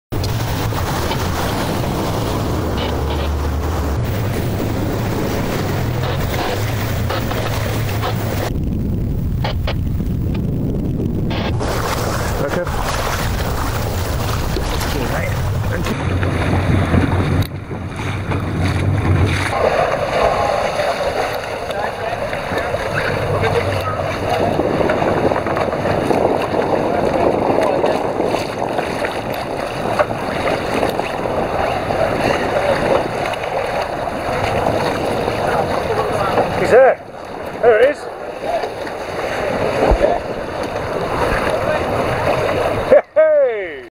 Dart and Salcombe lifeboats help rescue Humpback whale
Dart and Salcombe lifeboats were faced with an unusual casualty yesterday, when they were called to help an 8 metre and 15 tonne Humpback whale trapped in fishing gear. The lifeboats helped two members from the British Divers Marine Life Rescue organisation get to the whale and untangle her. Once freed the whale sped off.